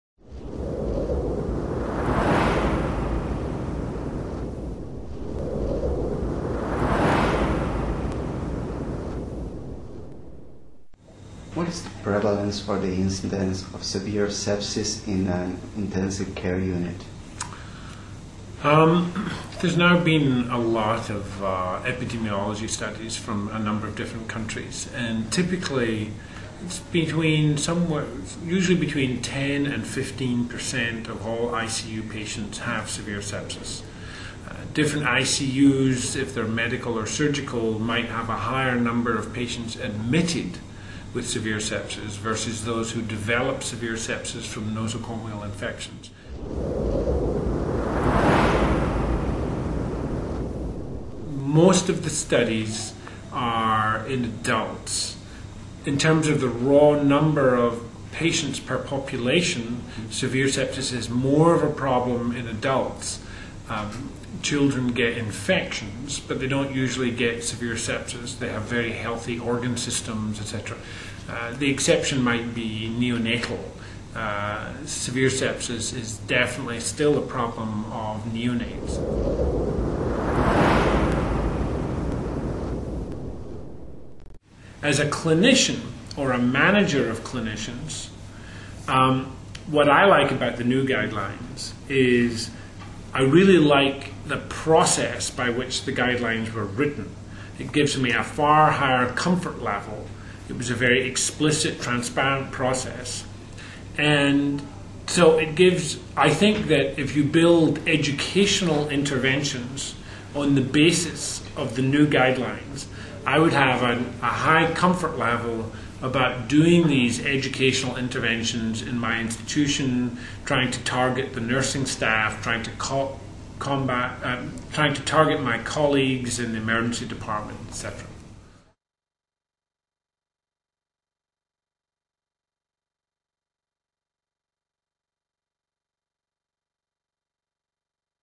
Nuevas Guí­as para Sepsis Severa [Subtitulado ESP]
Subtítulos disponibles en Español y Portugués.

Entrevistado: Derek C. Angus, M.D., M.P.H.
Nuevas Guí­as para Sepsis Severa
Especialidad: Terapia Intensiva

Sinopsis:

Entre el 10 y el 15 por ciento de todos los pacientes de la Unidad de Cuidados Intensivos (UCI) tienen sepsis severa. Esto depende de que la UCI sea médica o quirúrgica. También del número de pacientes admitidos con sepsis severa, versus aquellos que desarrollan una sepsis severa a partir de infecciones nosocomiales y de la edad de los pacientes (adultos, niños o neonatos).
En 2008, se publicó una nueva guía en el Intensive Care Medicine. Entre las diferencias que fueron introducidas se destacan el proceso de generación de las guías. También surgieron nuevas evidencias, que quizás no eran tan confiables. Entre ellas, la relacionada con la proteína C activada y el uso de esteroides.